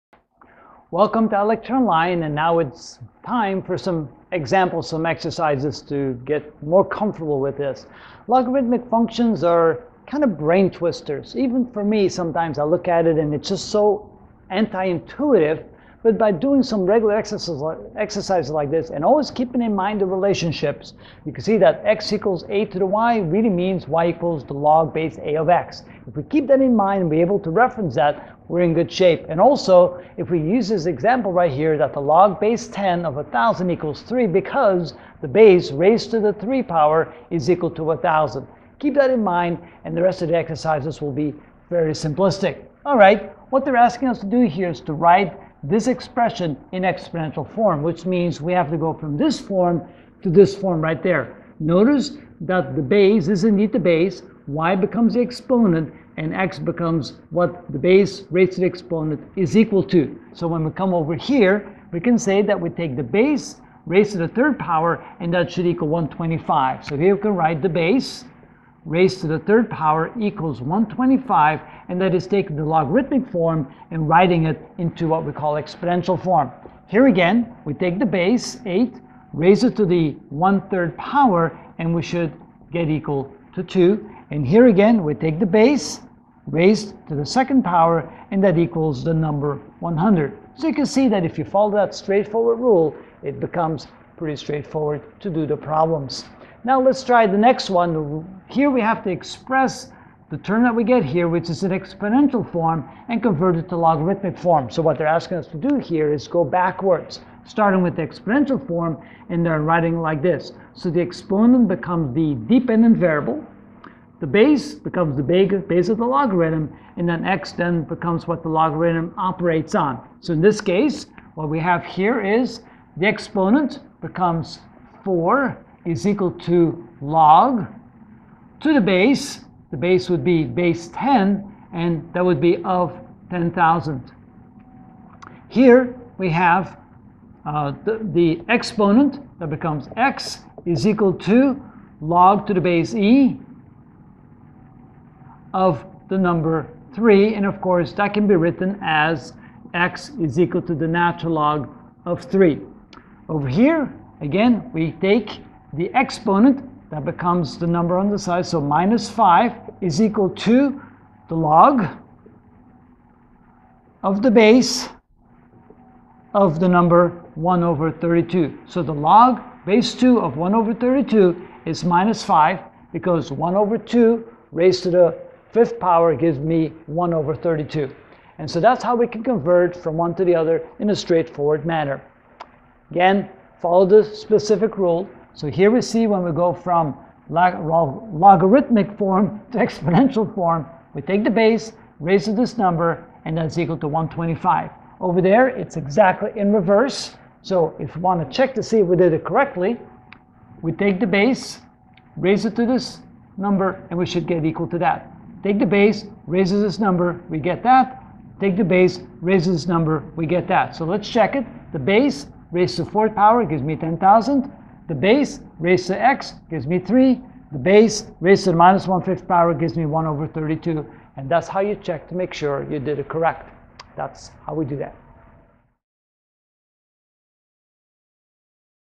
PreCalculus - Logarithmic Function (6 of 23) Logarithmic Function: Example 1
In this video I will write logarithmic expressions in exponential forms and expresses others in logarithmic forms.